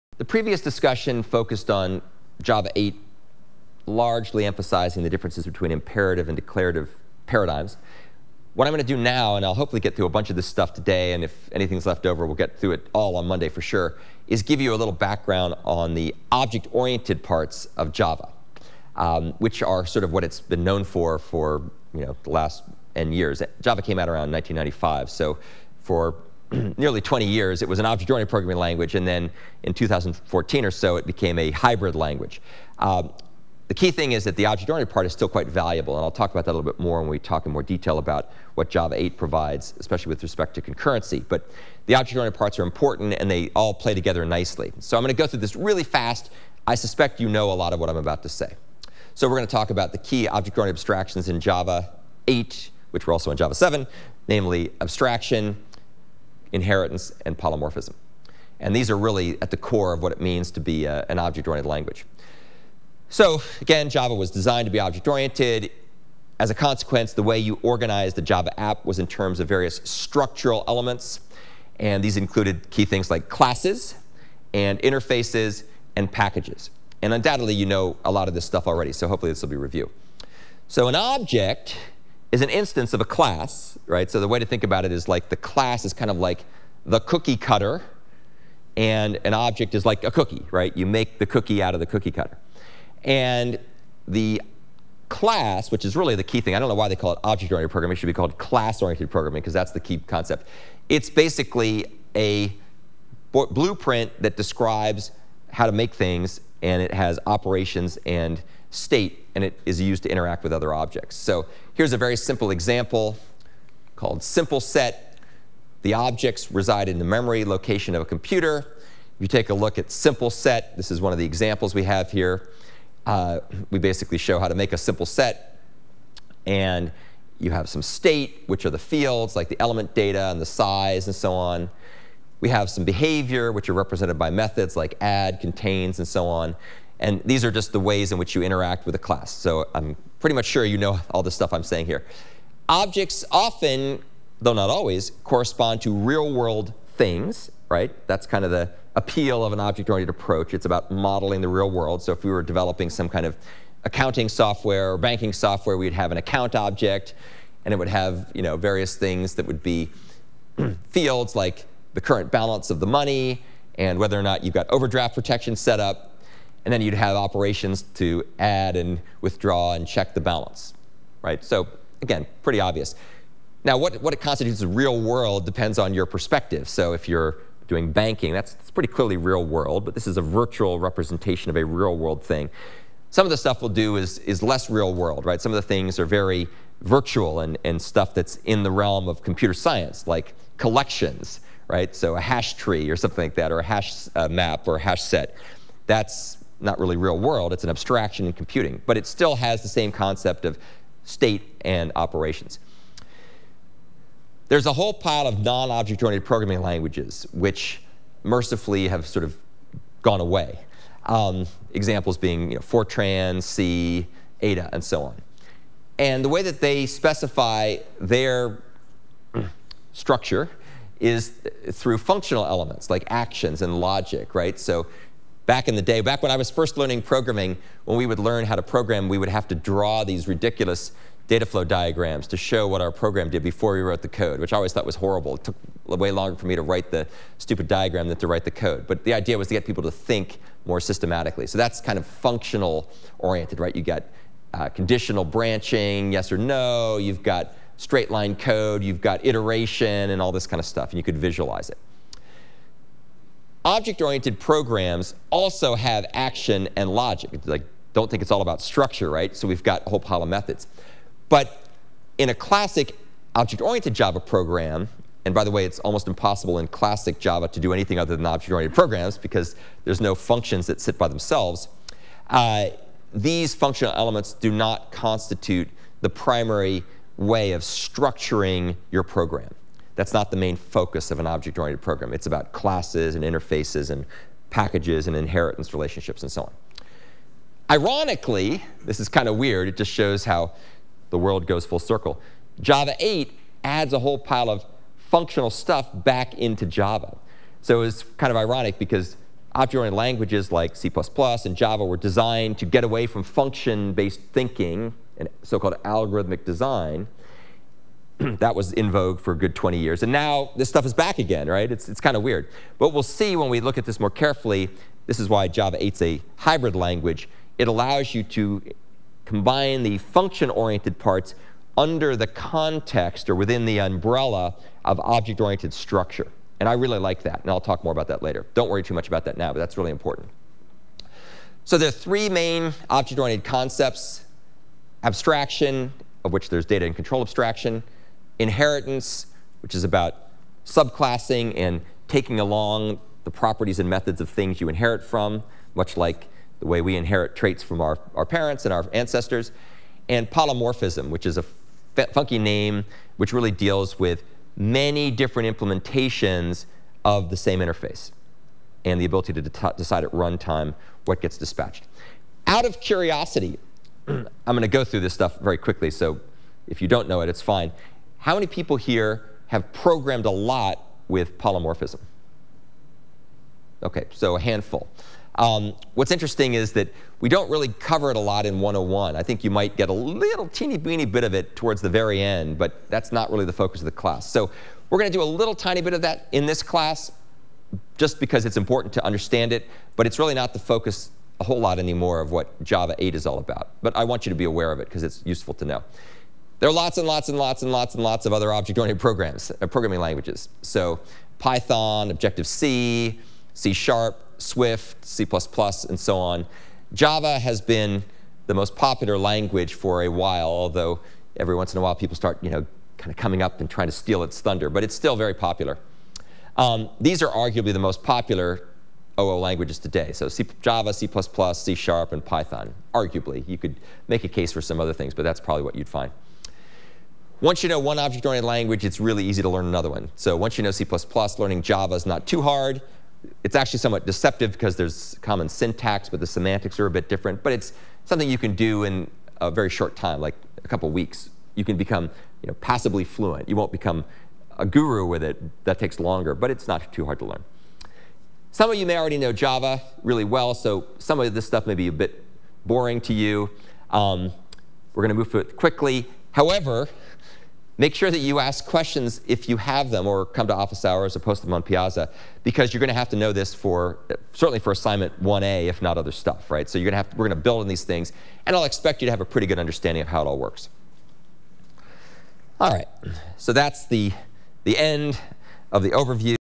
Overview of Java 8 Object-Oriented Programming Language Concepts
This video gives an overview of Java 8's object-oriented features, including data/control abstraction, inheritance, and polymorphism.